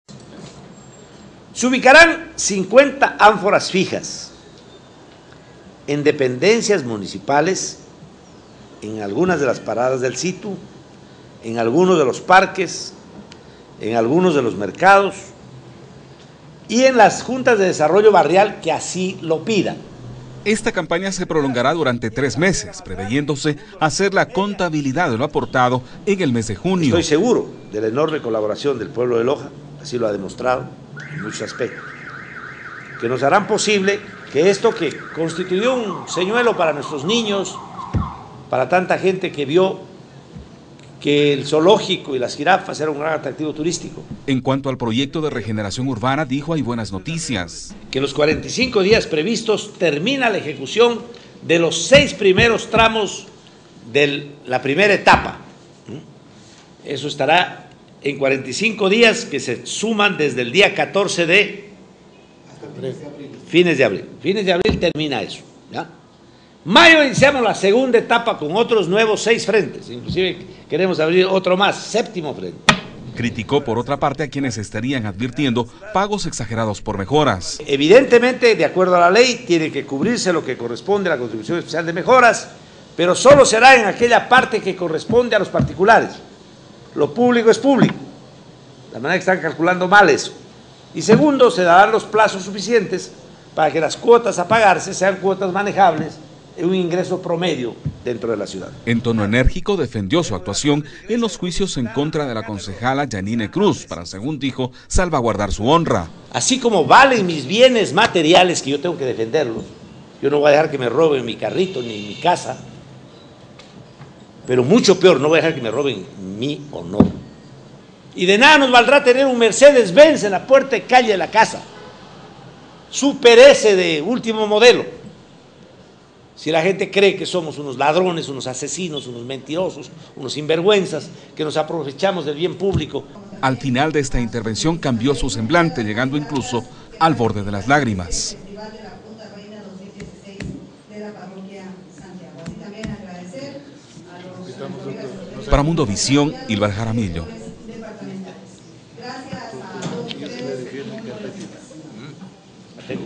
Alcalde de Loja se refirió a quienes buscan dañar su honra
Según lo informó en su conferencia de prensa semanal el Alcalde de Loja José Bolívar Castillo, una vez más cuestionó enérgicamente llegando incluso al borde de las lágrimas a quienes dijo buscan dañar su honra, por otra parte informo que  el cronograma de los trabajos del proyecto de regeneración urbana se está cumpliendo sin inconvenientes. En otro tema anunció que el próximo 3 de abril se lanzará oficialmente la colecta denominada JIRAFATÓN con la que se buscará financiar la adquisición de 2 jirafas para el zoológico de la ciudad.